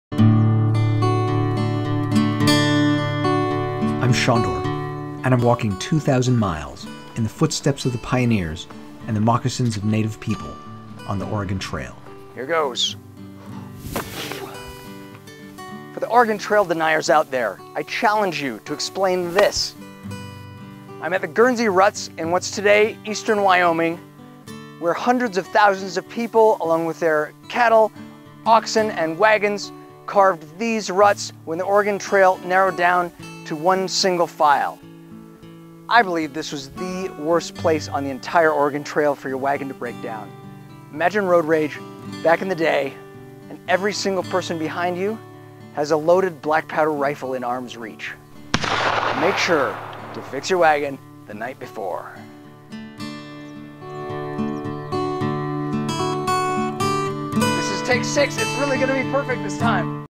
Sándor's Oregon Trail at the Guernsey Ruts, Wyoming
Oregon Trail deniers, I challenge you to explain this. Hundreds of thousands of pioneer emigrants cut the Guernsey Ruts in the rock with their wagon wheels in what's today Eastern Wyoming. Adventurer and writer Sándor Lau explores the ruts and tells why this is the worst place to break down because road rage was different when every wagon behind you had a loaded black powder rifle in arm's reach.